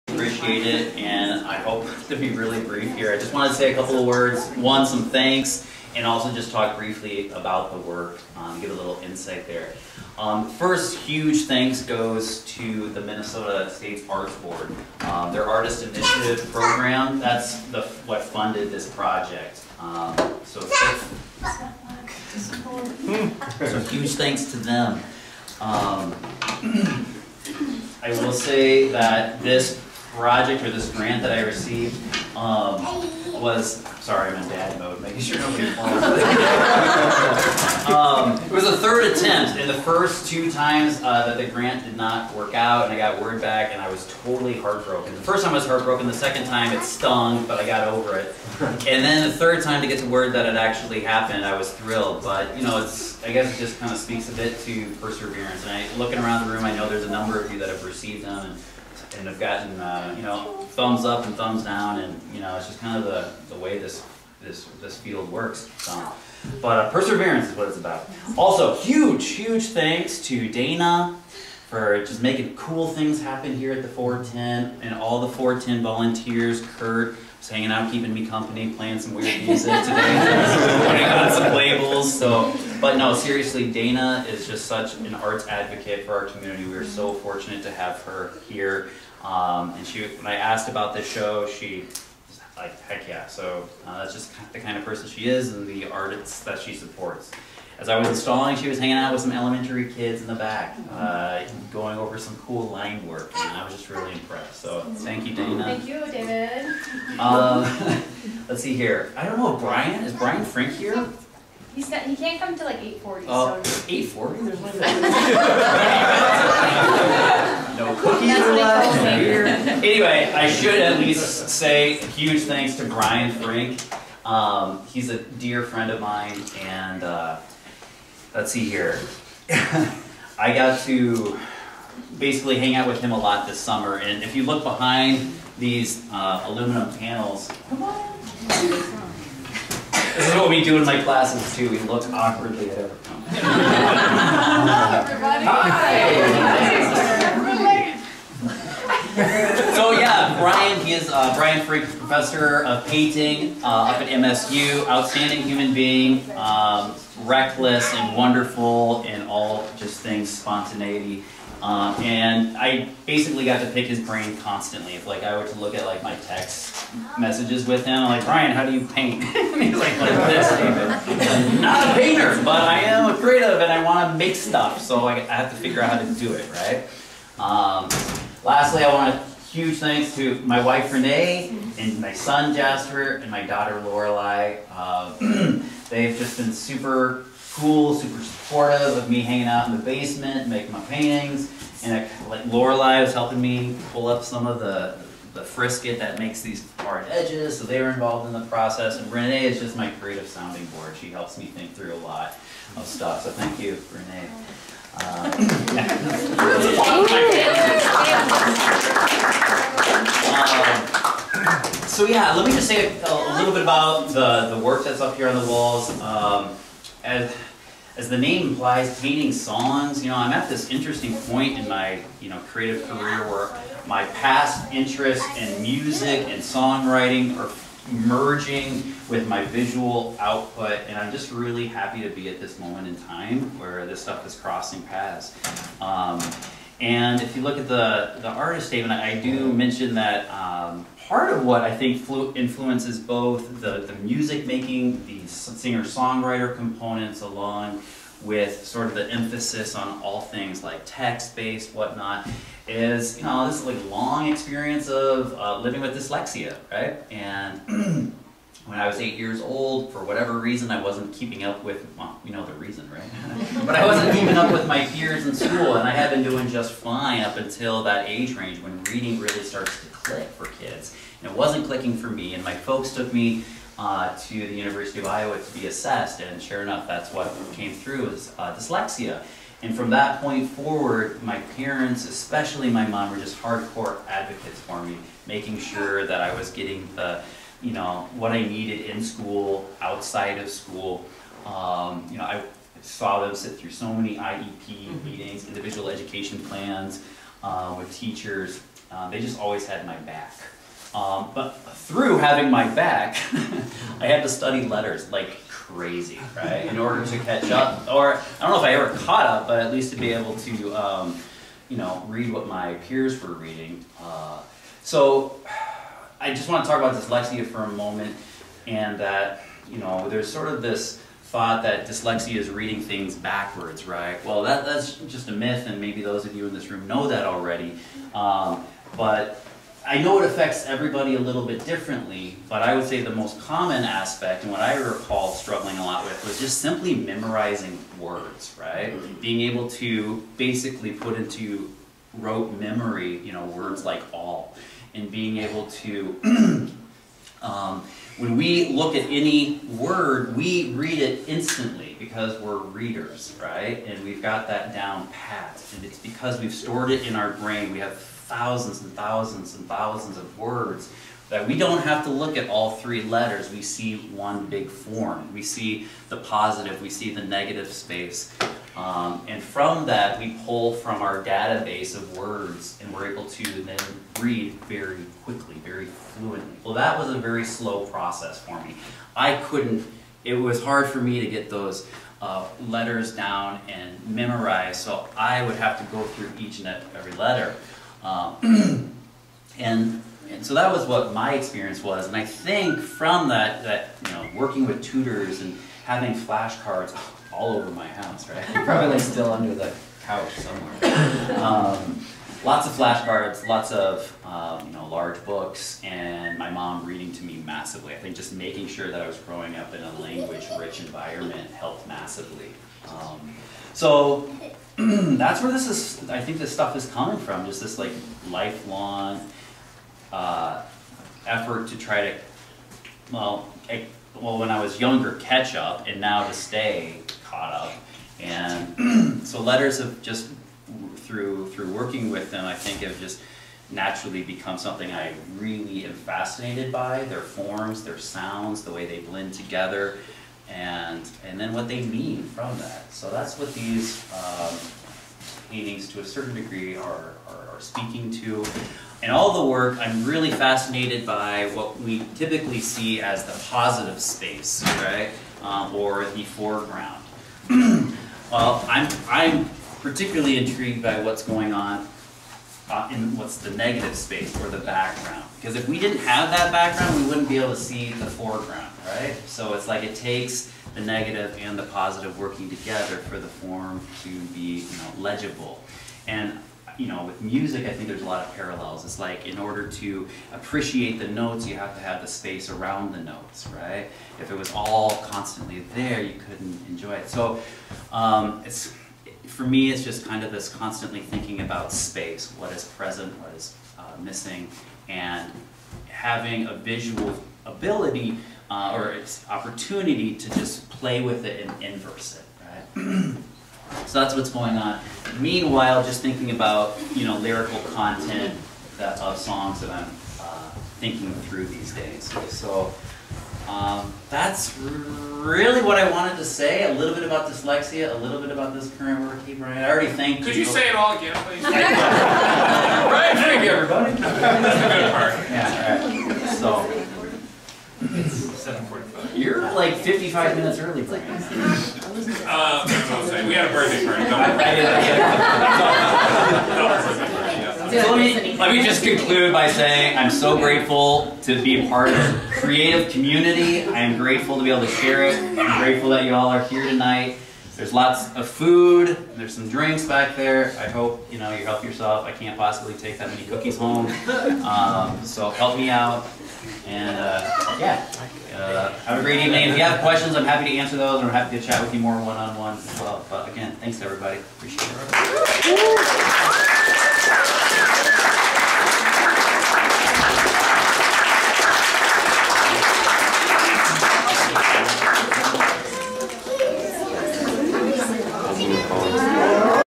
David Rogers - Painting Songs - January 17, 2020
David Rogers presents his installation Painting Songs 410 Project Mankato. January 17th, 2020.